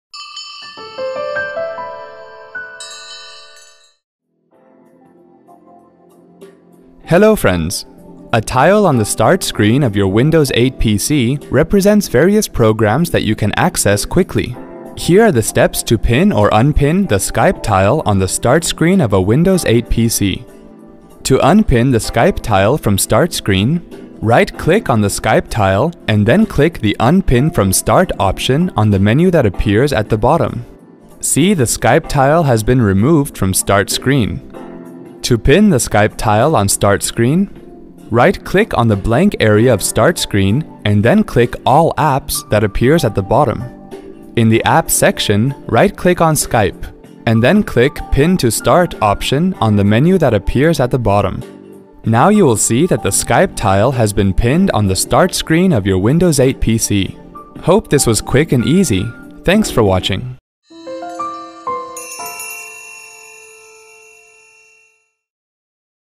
Pin and Unpin Skype® Tile from Start Screen on Windows® 8 PC
If you want to pin and unpin Skype® tile on the 'Start' screen of a Windows® 8 based PC, watch this video.